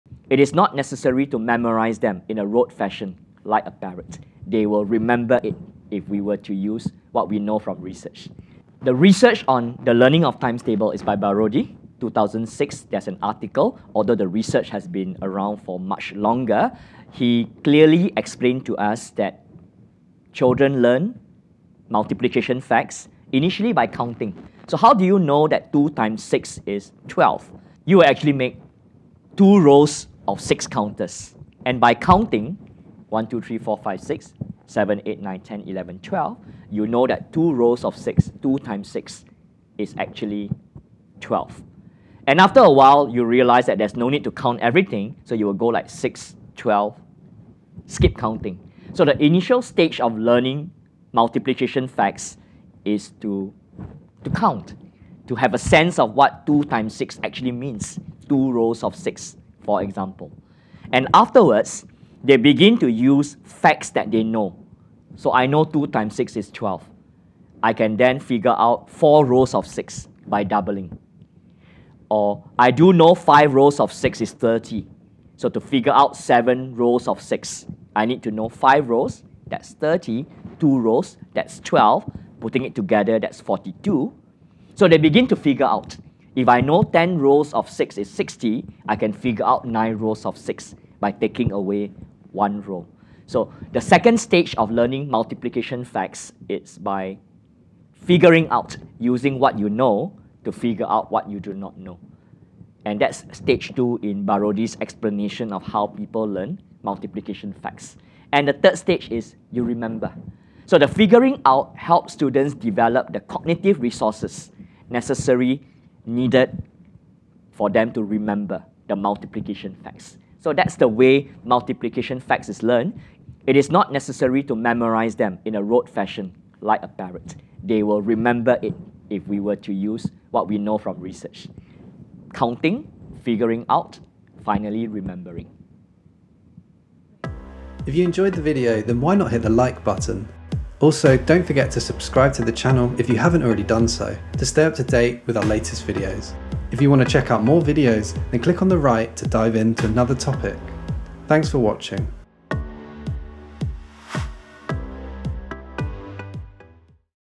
How Children Learn Multiplication Facts
Educational specialist and series consultant, Dr Yeap Ban Har, discusses how young learners acquire multiplication skills as researched and documented by such academics as Ziad Baroudi on which parts of the Maths — No Problem! multiplication curriculum is based.